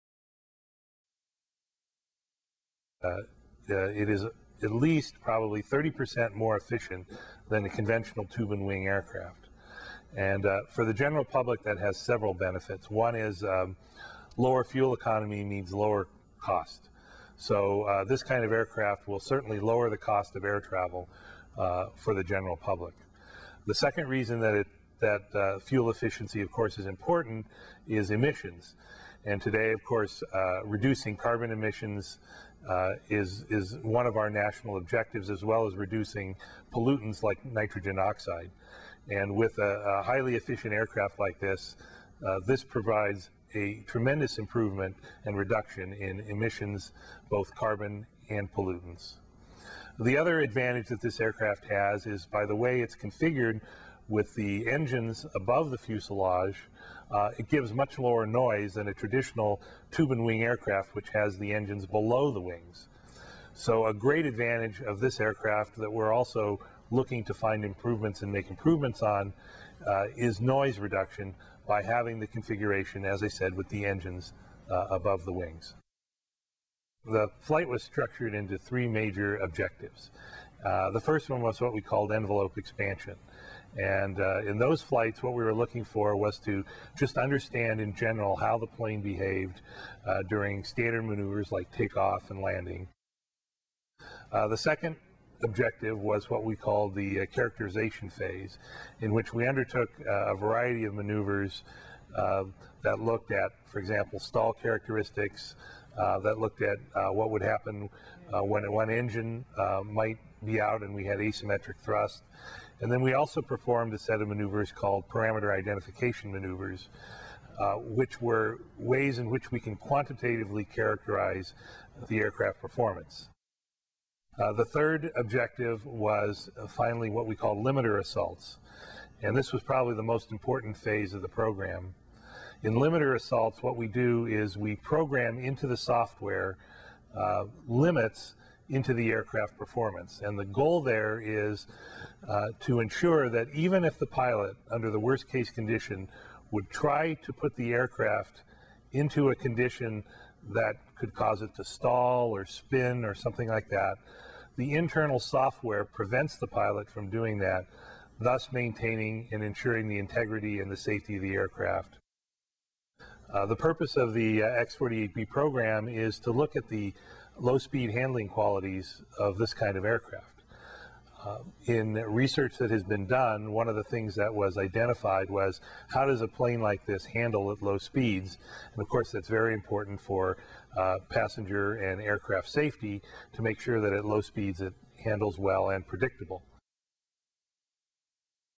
X-48B Project Manager Tim Risch Comments on the First Phase Flight Tests
This 3 minute and 9 second movie clip has X-48B Project Manager Tim Risch's comments on the first phase flight

Boeing Phantom Works has partnered with NASA and the Air Force Research Laboratory to study the structural, aerodynamic and operational advantages of the Blended-Wing Body advanced aircraft concept, a cross between a conventional plane and a flying wing design. The Air Force has designated the prototype the X-48B based on its interest in the design's potential as a multi-role, long-range, high-capacity military transport aircraft.